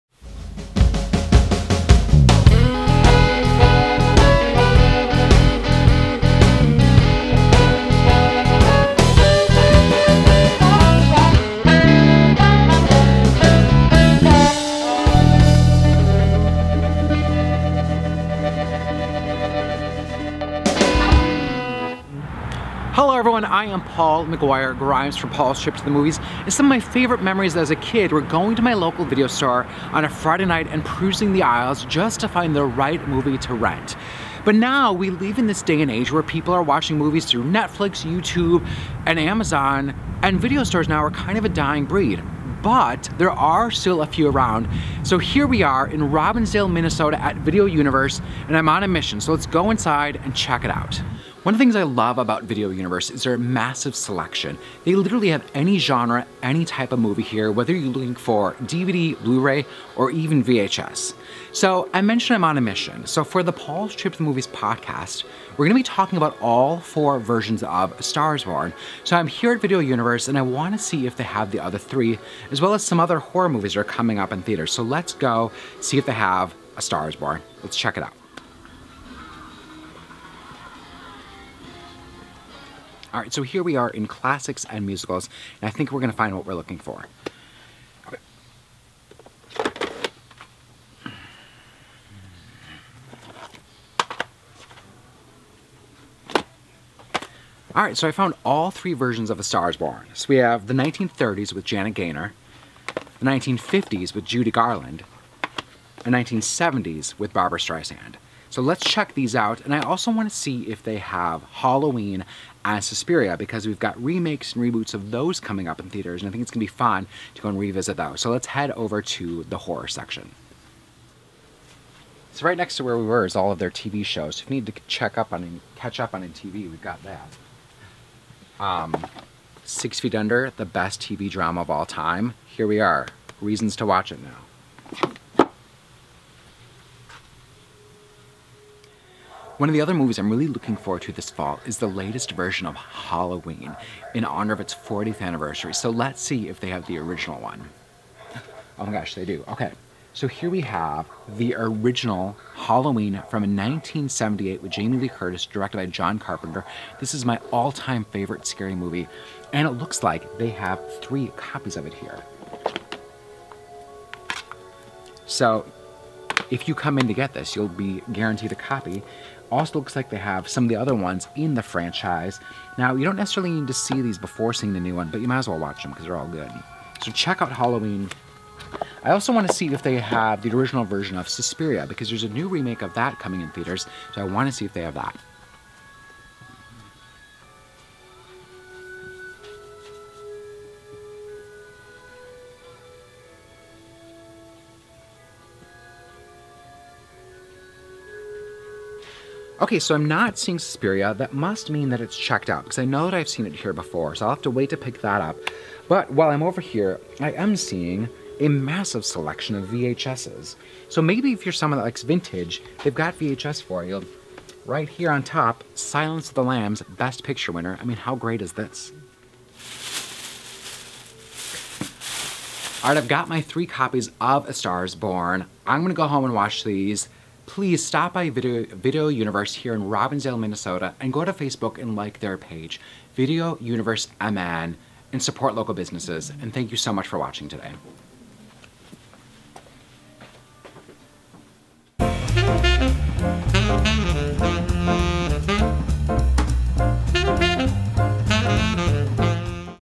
Paul's Trip to Video Universe
Video stores are a dying breed in this age of streaming services like Netflix, Hulu, and Amazon. Not to worry as there are a few left. I stopped by Video Universe in Robbinsdale, MN as I'm on a mission to find the three previous versions of "A Star is Born". Support local businesses and check out Video Universe. I know you'll come out with something to rent.
Photography/Editing: Adam McGuire